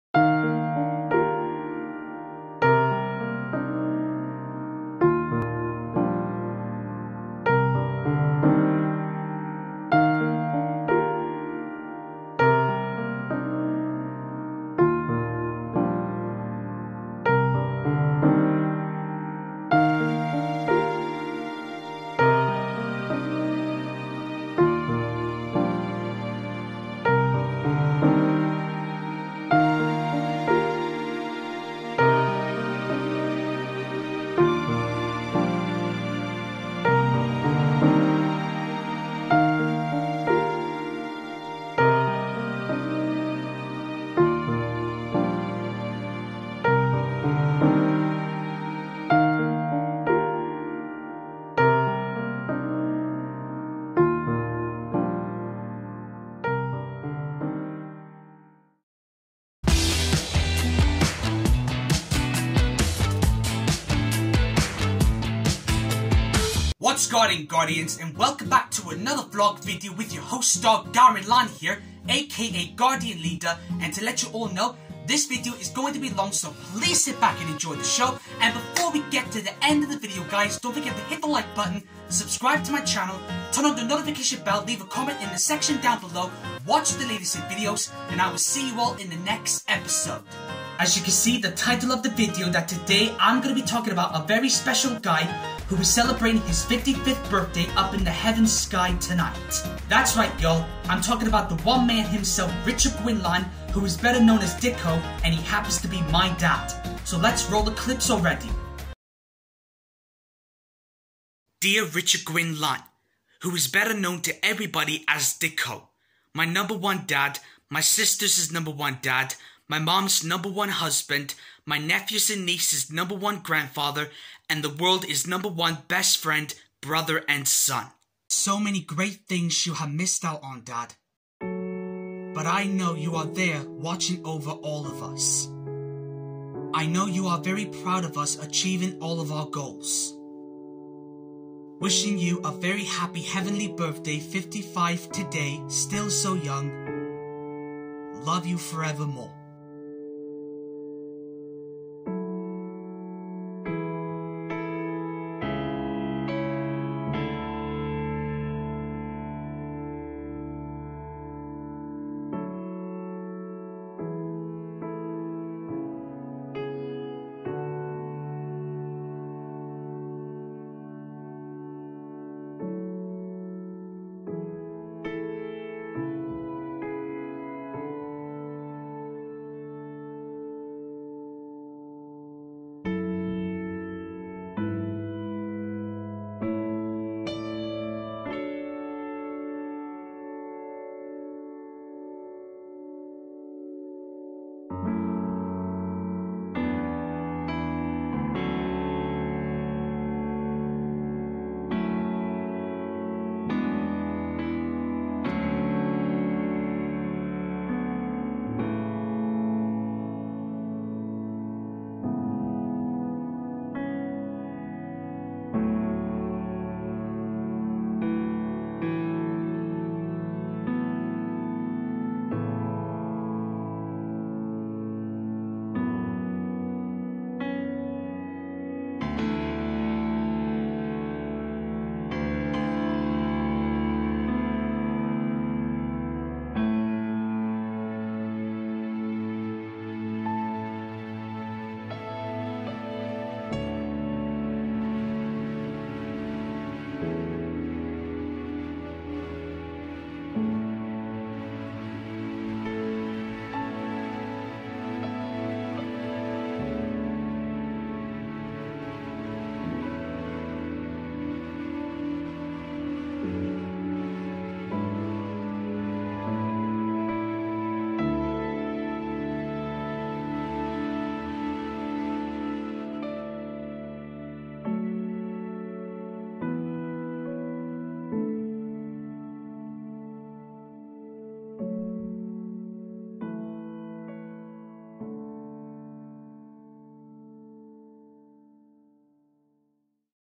Happy Heavenly Birthday Dad!
This is a very special tribute video for my dad because today is his 55th heavenly birthday miss him every day and night!

Gar!n.